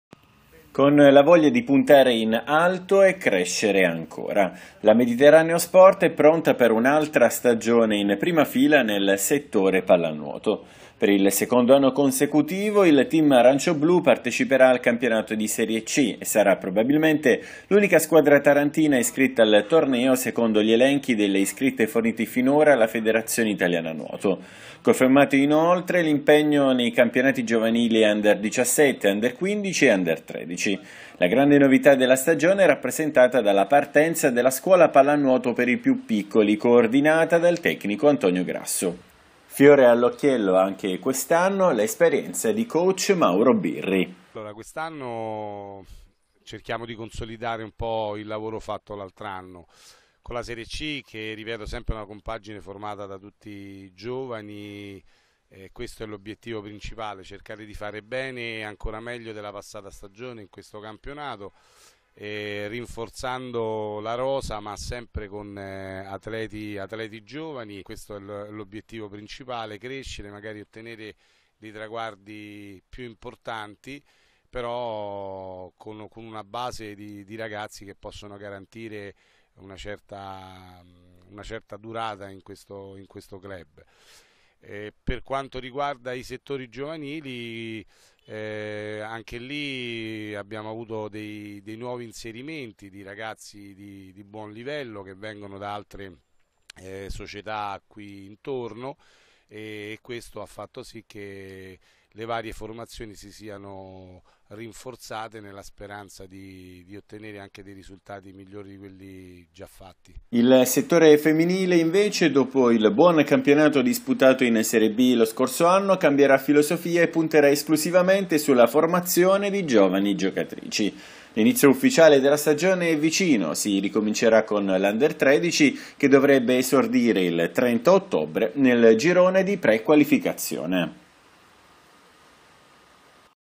Pallanuoto: Mediterraneo Sport, voglia di puntare in alto
Con la voglia di puntare in alto e crescere ancora. La Mediterraneo Sport è pronta per un'altra stagione in prima fila nel settore pallanuoto. La compagine jonica ha presentato stamattina i programmi della nuova stagione presso la piscina comunale di via Bruno.

Presenti, oltre al direttore sportivo Massimo Donadei, anche il responsabile tecnico Mauro Birri e l'assistant coach Mattia Di Noi.

Per il secondo anno consecutivo il team arancioblù parteciperà al campionato di serie C (sarà, probabilmente, l'unica squadra tarantina iscritta al torneo secondo gli elenchi delle iscritte forniti finora dalla Federazione Italiana Nuoto). Confermato, inoltre, l'impegno nei campionati giovanili under 17, under 15 e under 13. La grande novità della stagione è rappresentata dalla partenza della Scuola Pallanuoto per i più piccoli, coordinata dal tecnico Antonio Grasso.

«Per una società attenta al vivaio - sottolinea il ds Donadei - è fondamentale puntare su una Scuola Pallanuoto ben radicata e strutturata. Dopo dieci anni siamo riusciti a far partire il progetto e ne siamo felici. La bontà del lavoro compiuto nelle ultime stagioni è unanimemente riconosciuta: da sei anni siamo inseriti stabilmente nelle prime tre società di Puglia e ne siamo orgogliosi, vista la qualificata concorrenza. I sacrifici sono tanti e vengono ripagati: otterremo, infatti, due targhe speciali, per le giovanili maschili e l'under 17 femminile, nel corso del Gran Gala della Federnuoto che si svolgerà a Bari domenica 23 ottobre».

Le ambizioni non mancano. «Nel torneo di C - prosegue - puntiamo a migliorare il quarto posto della scorsa annata. Affronteremo il torneo con una squadra giovanissima, composta da diciassettenni, rinforzata dal nostro Gianluca Busco, portiere di lungo corso. E' il nostro Buffon. Riavremo, inoltre, con noi Davide Conte, tarantino doc che lo scorso anno ha contribuito alla promozione in A2 della Waterpolo Bari.  Riguardo alle squadre giovanili, speriamo di salire sempre più in alto».

Auspicio confermato dal tecnico Birri. «Proseguiremo il percorso di crescita - spiega - intrapreso nella stagione scorsa. L'under 17 ha sicuramente qualche carta da giocare in più: ci piacerebbe passare il turno per raggiungere le fasi nazionali. Per la squadra di serie C, invece,  stiamo valutando la collaborazione con varie società importanti., soprattutto con la Waterpolo Bari. Alcuni loro giovani atleti, non ancora pronti per il debutto in A2, potrebbero venire a darci una mano. La serie B? E' una prospettiva per il futuro: adesso preferiamo costruire un gruppo forte che si consolidi nel tempo».

Il settore femminile, invece, dopo il buon campionato disputato in serie B lo scorso anno, cambierà filosofia e punterà esclusivamente sulla formazione di giovani giocatrici.

L'inizio ufficiale della stagione è vicino: si ricomincerà con l'Under 13, che dovrebbe esordire il 30 ottobre nel girone di pre-qualificazione.